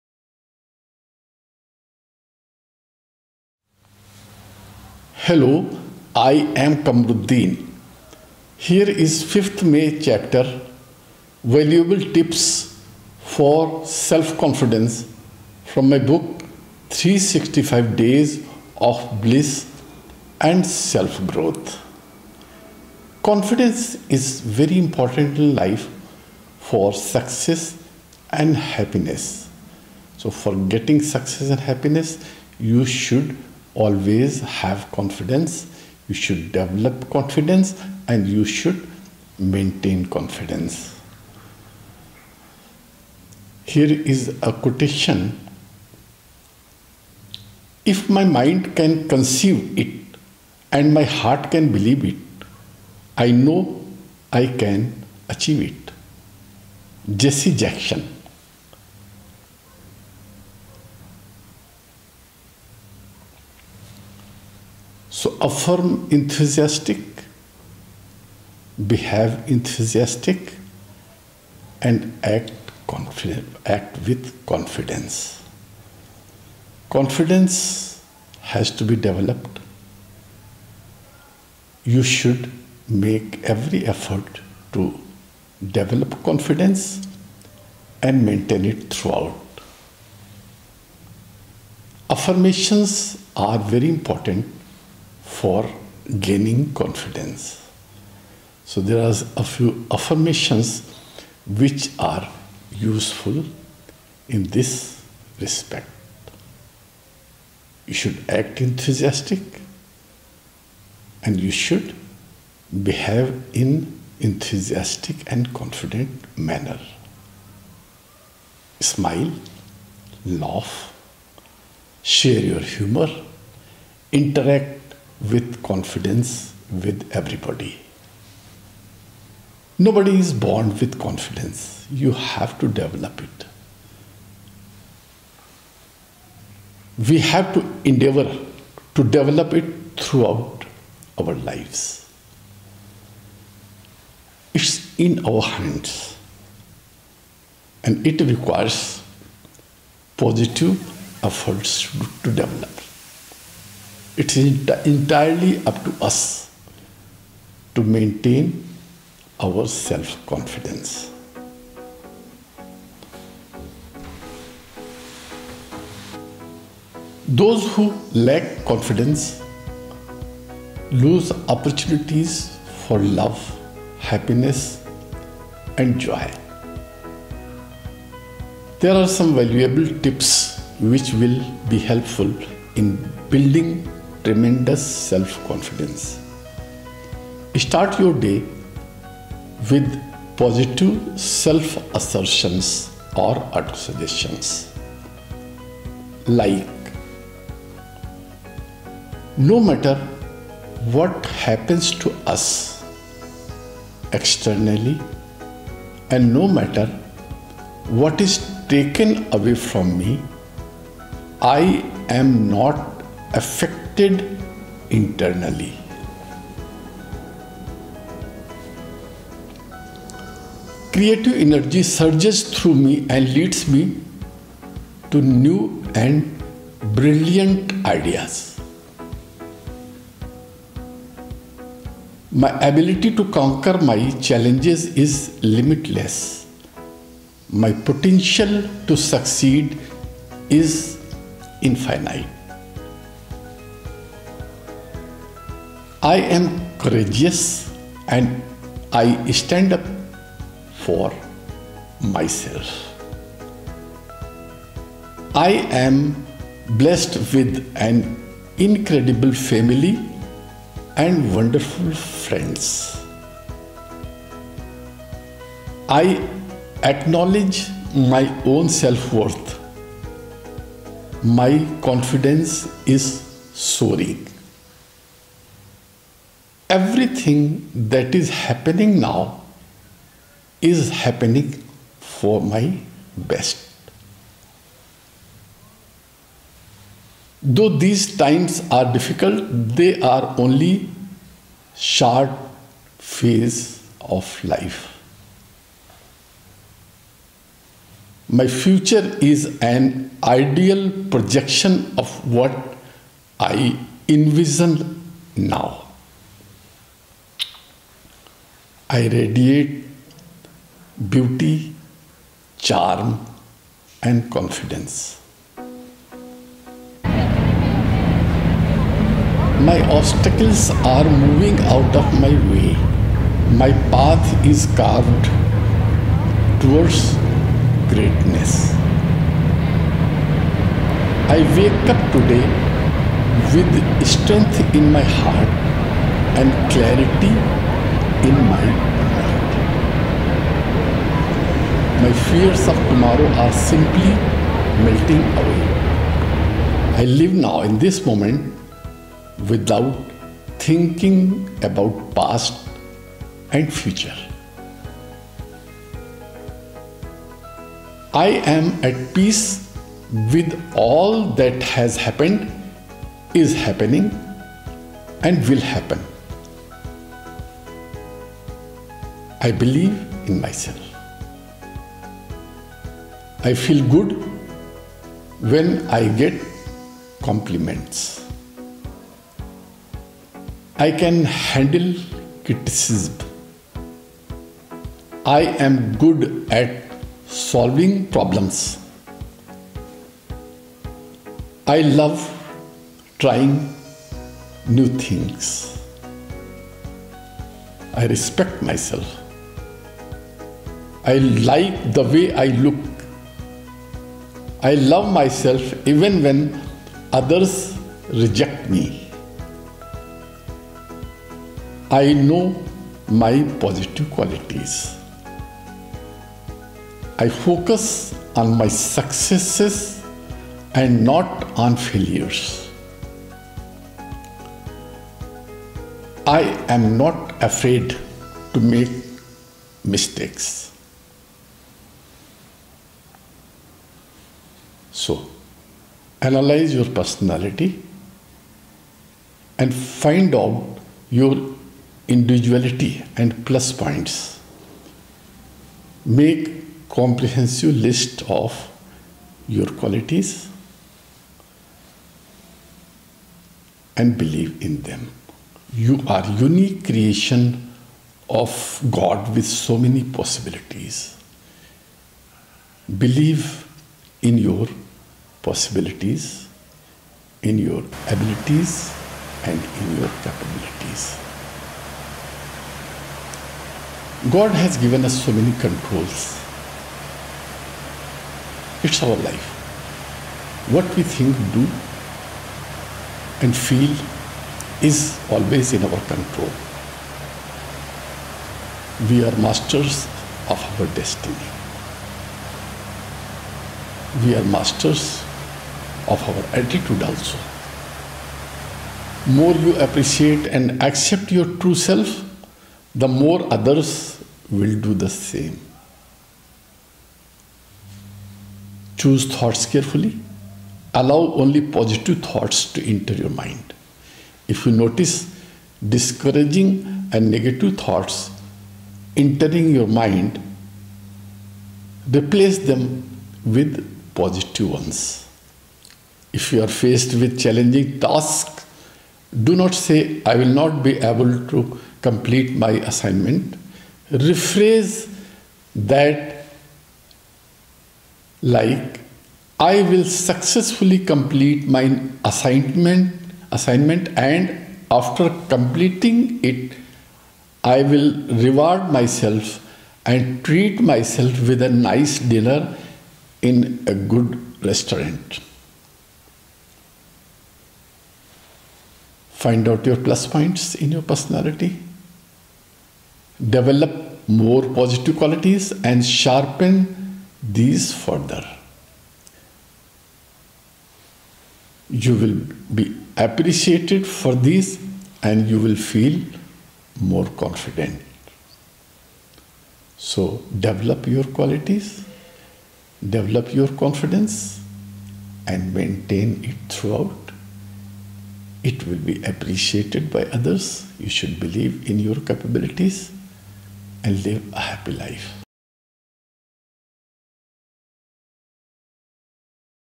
5th May -Valuable Tips for Boosting Self-Confidence - 365 Days of Bliss and Self-Growth
“If my mind can conceive it, and my heart can believe it, I know I can achieve it.” ~ Jesse Jackson
Confidence plays role in achieving success and happiness. Have, therefore, confidence in yourself. Affirm, act enthusiastically and be happy. Your body language and handshake should ooze self-confidence. Smile, laugh, share and show your happiness.
Nobody is born with confidence. We have to endeavour to develop it throughout our lives. It is in our hands, and it requires positive efforts to develop. It is entirely up to us to maintain our own self-confidence. Those who lack self-confidence lose opportunities for love, happiness and joy.
There are some valuable tips, which will be helpful in building tremendous self-confidence. Start your day with positive self-assertions and auto suggestions like:
• No matter what happens externally, and no matter what’s taken away from me, I’m not affected internally.
• Creative energy surges through me and leads me to new and brilliant ideas.
• My ability to conquer my challenges is limitless; my potential to succeed is infinite.
• I am courageous, and I stand up for myself.
• I am blessed with an incredible family and wonderful friends.
• I acknowledge my own self-worth; my confidence is soaring.
• Everything that is happening now is happening for my ultimate good.
• Though these times are difficult, they are only a short phase of life.
• My future is an ideal projection of what I envision now.
• I radiate beauty, charm, and grace.
• My obstacles are moving out of my way; my path is carved towards greatness.
• I wake up today with strength in my heart and clarity in my mind.
• My fears of tomorrow are simply melting away. I live now in this moment without thinking about past and future.
• I am at peace with all that has happened, is happening, and will happen.
• I feel good when I get compliments.
• I can handle criticism.
• I am good at solving problems.
• I love trying new things.
• I respect myself.
• I like the way I look.
• I love myself even when others reject me.
• I know my positive qualities.
• I focus on my successes and not my failures.
• I’m not afraid to make mistakes.
Analyse your personality and find out your individuality and plus points. Make a comprehensive list of all your positive traits and achievements. You are a unique creation of God with immense possibilities. God has given us so many controls. It is our life. What we think, do and feel is in our control. We are masters of our destiny. We are masters of our attitude also. The more you appreciate and accept your true self, the more others will do the same.
Choose thoughts carefully and allow only positive thoughts to enter your mind. If you notice discouraging and negative thoughts, replace them with positive ones. If you are faced with a challenging task, do not say: "I will not be able to complete my assignment.” Rephrase that like: "I will successfully complete my assignment and after completing it, I will reward myself and treat myself with a nice dinner in a good restaurant.”
Find out your plus points in your personality. Develop more positive qualities and sharpen these further, which you already have. You will be appreciated for these and you will feel confident.